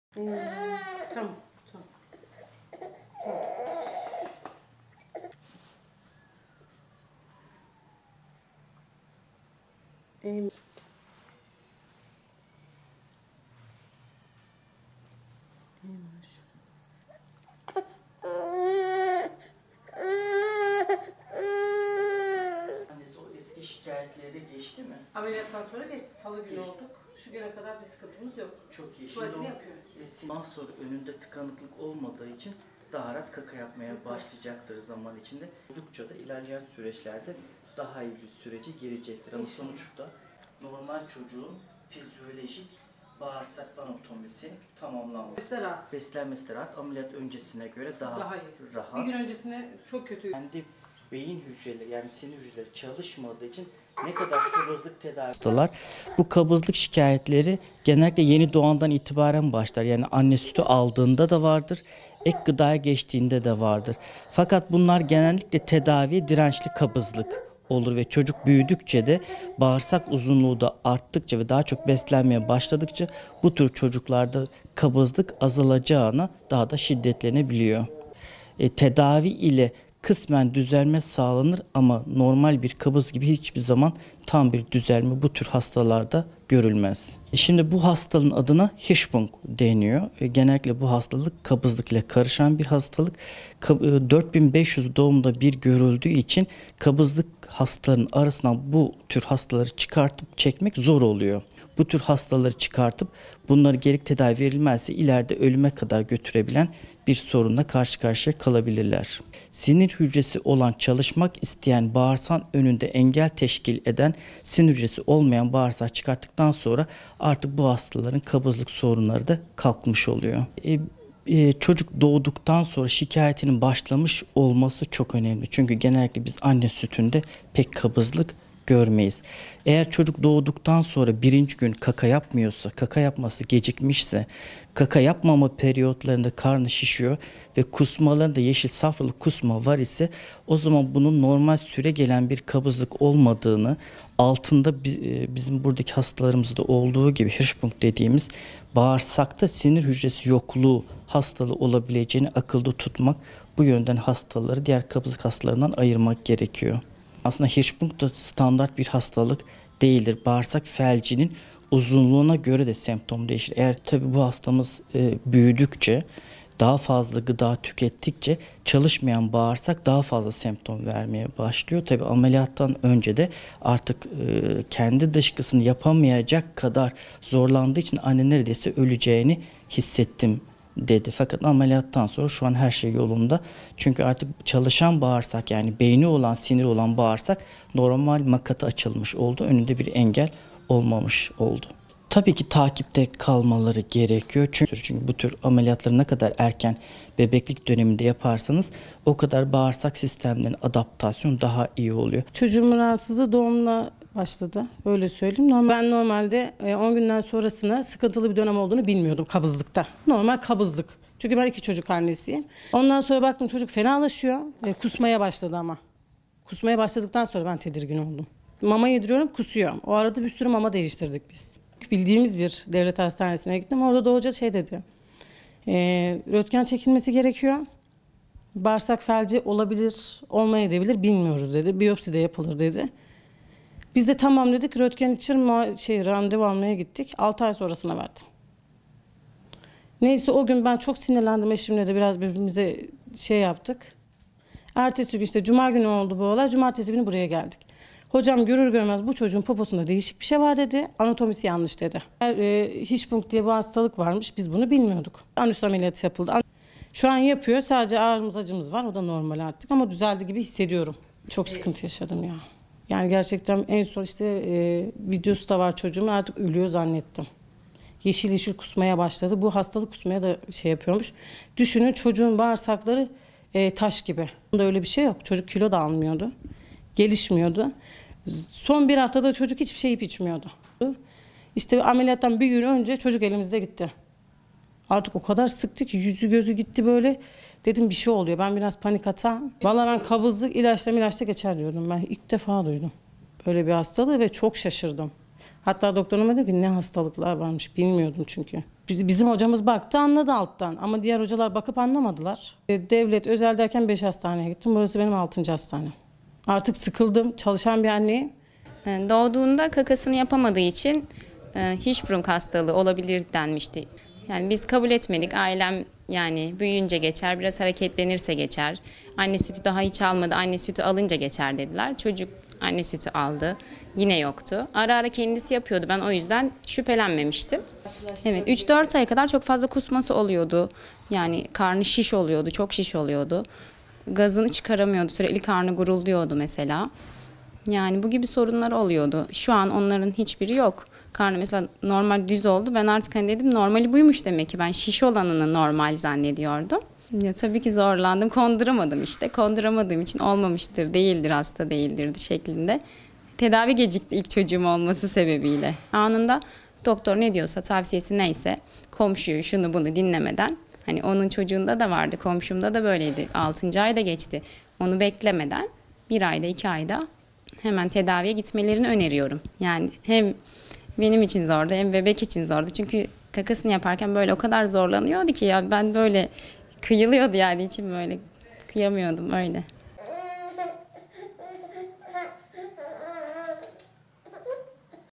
Bebeklerde bağırsak felcine dikkat: "Bebeğim ölüyor zannettim"
Çocuklarda doğumsal rahatsızlıklar arasında yer alan bağırsak felci olarak bilinen “hirschsprung”un çoğu zaman kabızlık ile karıştırıldığını anlatan uzmanlar uyarıyor. Bağırsak felci nedeniyle zor günler geçirdiklerini anlatan anne Çiler Doğan, “Kabızlık dedim, ilaçla geçer sanıyordum ilk defa duydum böyle bir hastalığı, çok şaşırdım. Çok sıkıntı yaşadım artık ölüyor zannettim, düşünün, çocuğun bağırsakları taş gibi, kilo da almıyor, gelişmiyordu, şu an iyi ” dedi.
Çocuklarda doğumsal rahatsızlıklar arasında yer alan, bağırsağın sinir sistemini ilgilendiren “Hirschsprung” yani “bağırsak felci” bebeğin dünyaya gözlerini açtığı andan itibaren karın şişliği, dışkı yapamama gibi şikayetlerle kendini gösteriyor. Uzmanlar dışkı yapılamaması durumunun aileler tarafından geçici ve kabızlık olarak düşünüldüğünü ancak bağırsak felcine karşı dikkat edilmesi gerektiği noktasında uyarıyor. Çocuk Cerrahisi Uzmanı Doç. Dr. Süleyman Çelebi de 11 aylık bebeklerinin önce kabız olduğunu düşünen ardından çeşitli hastanelere götürdüklerini ifade eden iki ailenin bebeklerine uygulanan tedaviyi anlattı. Doç. Dr. Çelebi, hastalığa karşı ailelere önemli uyarılarda bulundu.

“Ölüme kadar götürebilen bir sorunla karşı karşıya kalabilirler”
Hastalığın kabızlıkla karıştırıldığı için kimi zaman tedavisinde büyük gecikmeler yaşandığına dikkat çeken ve ailelere uyarılarda bulunan Çocuk Cerrahisi Uzmanı Doç. Dr. Süleyman Çelebi, “Kabızlık şikayetleri genellikle yeni doğandan itibaren başlar yani anne sütü aldığında da vardır, ek gıdaya geçtiğinde de vardır. Fakat bunlar genellikle tedaviye dirençli kabız olur ve çocuk büyüdükçe, bağırsak uzunluğu arttıkça ve daha çok beslenmeye başladıkça bu tür çocuklarda kabızlık azalacağına daha da şiddetlenebiliyor. Tedavi ile kısmen düzelme sağlanır ama hiçbir zaman tam bir düzelme görülmez. Bu hastalığın adına hirschsprung deniliyor, genellikle bu kabızlık ile karışan bir hastalık. 4 bin 500 doğumda bir görüldüğü için kabızlık hastalarının arasından bu tür hastaları çıkartıp çekmek zor oluyor. Hastaları gerekli tedavi verilmezse ileride ölüme kadar götürebilen bir sorunla karşı karşıya kalabilirler. Sinir hücresi olan, çalışmak isteyen bağırsağın önünde engel teşkil eden sinir hücresi olmayan bağırsağı çıkarttıktan sonra artık bu hastaların kabızlık sorunları da kalkmış oluyor. Çocuk doğduktan sonra şikayetinin başlamış olması çok önemli çünkü genellikle biz anne sütünde pek kabızlık görmeyiz. Eğer çocuk doğduktan sonra birinci günü tuvaletini yapmıyorsa, yapması gecikmişse, dışkı yapmama periyotlarında karnı şişiyor ve yeşil safralı kusma var ise o zaman bunun normal süregelen bir kabızlık olmadığını bağırsakta sinir hücresi yokluğu hastalığı olabileceğini akılda tutmak bu yönden hastaları diğer kabızlık hastalarından ayırmak gerekiyor. Aslında hirschsprung da standart bir hastalık değildir, bağırsak felcinin uzunluğuna göre de semptom değişir. Bu hastamız büyüdükçe, daha fazla gıda tükettikçe çalışmayan bağırsak daha fazla semptom vermeye başlıyor. Ameliyattan önce de artık kendi dışkısını yapamayacak kadar zorlandığı için 'Anne neredeyse öleceğini hissettim' dedi ama ameliyattan sonra şu an her şey yolunda. Beyni, siniri olan bağırsak normal makata açılmış oldu. Takipte kalmaları gerekiyor. Bu tür ameliyatları ne kadar erken yaparsanız bağırsak sistemlerine adaptasyonu o kadar iyi oluyor “şeklinde konuştu.

“Çok sıkıntı yaşadım artık ölüyor zannettim"
Doğum sonrası bebeğinin rahatsızlığı nedeniyle çok zor günler geçirdiklerini anlatan anne Çiler Doğan, “Çocuğumun rahatsızlığı doğumla başladı. Ben normalde 10 günden sonrasının kabızlıkta sıkıntılı olduğunu bilmiyordum. Baktım çocuk fenalaşıyor, kusmaya başladı, bundan sonra ben tedirgin oldum. Bildiğimiz bir devlet hastanesine gittim. Röntgen için randevu almaya gittik, 6 ay sonrasına verdi, ben çok sinirlendim. Başka bir hastaneye geldik, hirschsprung diye bir hastalık varmış. Anüs ameliyatı yapıldı, şu an yapıyor, sadece ağrımız, acımız var o da normal artık düzeldi gibi hissediyorum. Çok sıkıntı yaşadım artık ölüyor zannettim, yeşil yeşil kusmaya başladı. Düşünün, çocuğun bağırsakları taş gibi, çocuk kilo da almıyor, gelişmiyordu son 1 haftada çocuk hiçbir şey yiyip, içmiyordu. Ameliyattan bir gün önce çocuk elimizde gitti artık o kadar sıktı ki kendisini yüzü, gözü gitti bir şey oluyor dedim. Ben kabızlık dedim, ilaçla geçer sanıyordum ilk defa duydum böyle bir hastalığı ve çok şaşırdım. Hatta doktoruma dedim ne hastalıklar varmış, bilmiyordum. Bizim hocamız baktı anladı, alt taraftan ama diğer hocalar anlamadı. Devlet, özel derken 5
hastaneye gittim burası benim altıncı hastanem artık sıkıldım" dedi.